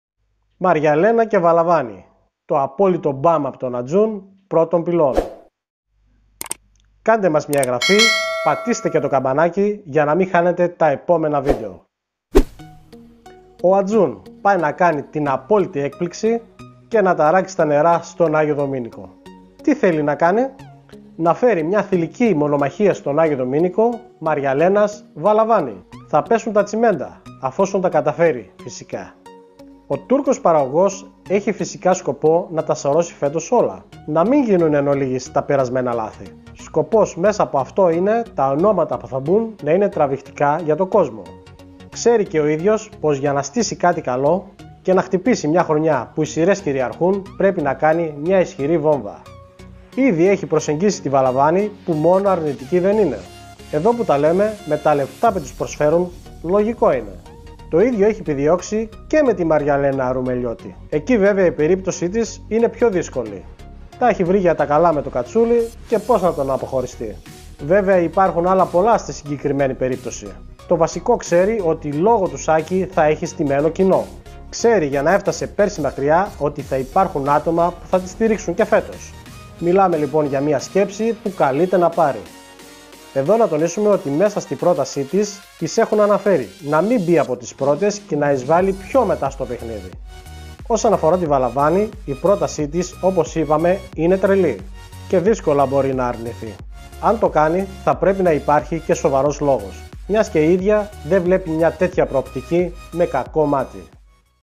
Survivor spoiler: Μαριαλένα και Βαλαβάνη… το απόλυτο ΜΠΑΜ από τον Ατζούν προ των πυλών!
Ο Ατζούν πάει να κάνει την απόλυτη έκπληξη και να ταράξει για τα καλά τα νερά στον Άγιο Δομίνικο. Τι θέλει να κάνει; Να φέρει μία Θυλική μονομαχία στην Δομινικανή Δημοκρατία. Μαριαλένα – Βαλαβάνη. Θα πέσουν τα τσιμέντα εφόσον το καταφέρει φυσικά.

Ο Τούρκος παραγωγός έχει σκοπό να τα σαρώσει φέτος όλα. Να μην γίνουν εν ολίγοις τα περσινά λάθη. Σκοπός μέσα από αυτό είναι τα ονόματα που θα μπουν να είναι τραβηχτά για τον κόσμο. Ξέρει και ο ίδιος πως για να στήσει κάτι καλό και να χτυπήσει μία χρονιά που οι σειρές κυριαρχούν πρέπει να κάνει μία γερή Βόμβα.

Ήδη έχει προσεγγίσει την Βαλαβάνη, η οποίο μόνο αρνητική δεν είναι. Εδώ που τα λέμε με τα λεφτά που τις προσφέρουν λογικό είναι. Μιλάμε για ποσό μαμούθ. Το ίδιο έχει επιδιώξει και με την Μαριαλένα Ρουμελιώτη. Εκεί βέβαια η περίπτωσή της είναι πιο δύσκολη. Τα έχει βρει για τα καλά με τον Κατσούλη και πως να τον αποχωριστεί; Βέβαια υπάρχουν πολλά αλλά στη συγκεκριμένη περίπτωση.

Το βασικό ξέρει, ότι λόγο του Σάκη θα έχει ήδη στημένο κοινό. Ξέρει ότι για να έφτασε πέρυσι μακριά έχει άτομα που θα την στηρίξουν και την φετινή χρονιά. Μιλάμε λοιπόν για μία σκέψη που καλείτε να πάρει. Εδώ να τονίσουμε ότι μέσα στη πρόταση τις έχουν αναφέρει να μην μπει από τις πρώτες και να εισβάλει πιο μετά στο παιχνίδι.

Όσον αφορά την Βαλαβάνη τώρα η πρόταση όπως είπαμε είναι τρελή. Δύσκολα μπορεί να αρνηθεί. Αν το κάνει θα πρέπει να υπάρχει και σοβαρός λόγος, μιας και η ίδια δεν βλέπει μία τέτοια προοπτική με κακό μάτι.

Είπαμε. Τα δύο κορίτσια έχουν δεχθεί προτάσεις, αλλά δεν έχουν συμφωνήσει ακόμα. Οι συζητήσεις συνεχίζονται. Σίγουρα τις επόμενες ημέρες, η το επόμενο διάστημα θα έχουμε κάτι πιο ζεστό στις κεραίες μας.

Μένει να φανεί οι προθέσεις τους και σε πιο επίσημο επίπεδο. Να γνωρίζουμε και όλοι μας τι μέλη γενέσθαι.

Πηγή φωτογραφιών : thrylos24.gr